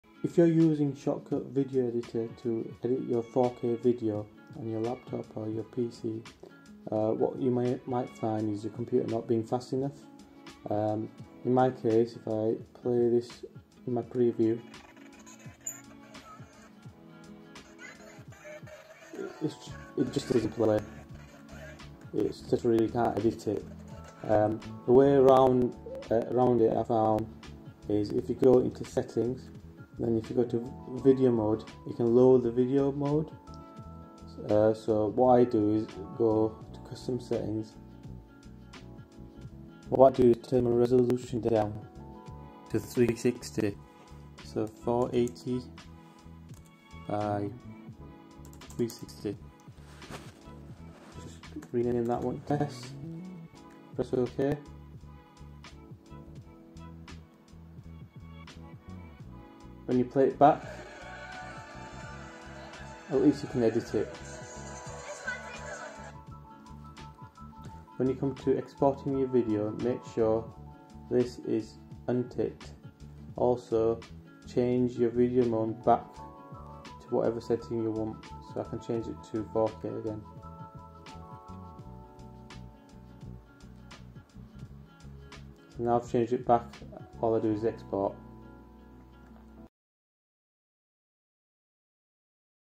Shotcut video editor - How to Reduce Preview Lag
Shotcut is a free and open-source cross-platform video editing application.

Problem was with my laptop is a little slow now. By changing the Video mode I have less lag on the video preview.

My Laptop Specs:
Acer E1-571
Intel i5-3230m CPU @ 2.60ghz
Ram 8GB
500gb SSD